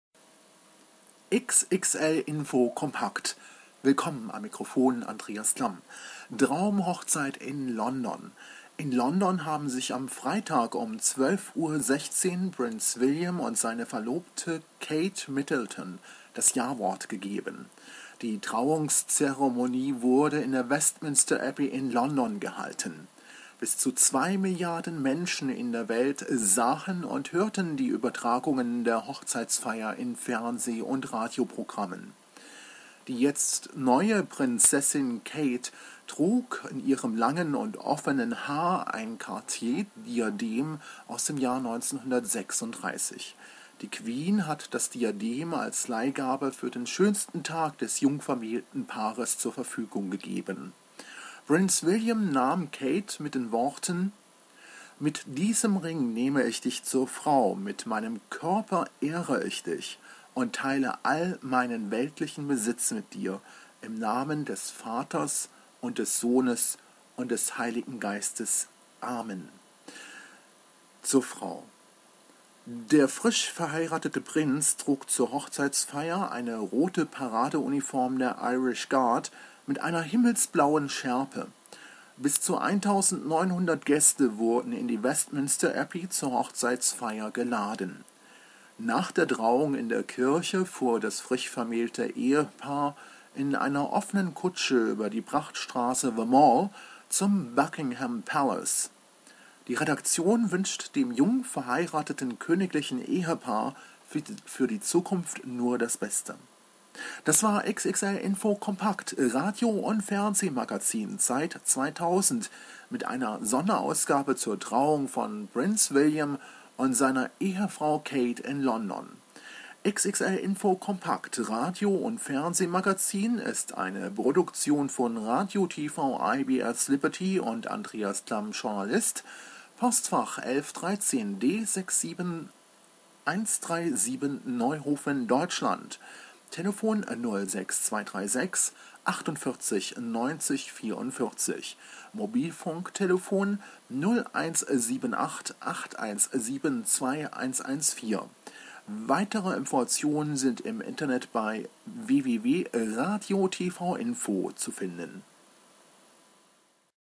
XXL-info. Compact. Hochzeit in London
Prinz William hat am Freitag seine Verlobte Kate Middleton zur Ehefrau genommen und geheiratet. XXL-info.Compact mit einem Bericht zur Hochzeit in London. Fotos, Produktion, Redaktion und Moderation: Andreas Klamm, Journalist, Radio TV IBS Liberty, und 3mnews.org, Nachrichtenagentur, CC 2011, Creative Commons Lizenz. Die Produktion darf weiter veröffentlicht und genutzt werden. Der Name des Produzenten sollte angegeben werden.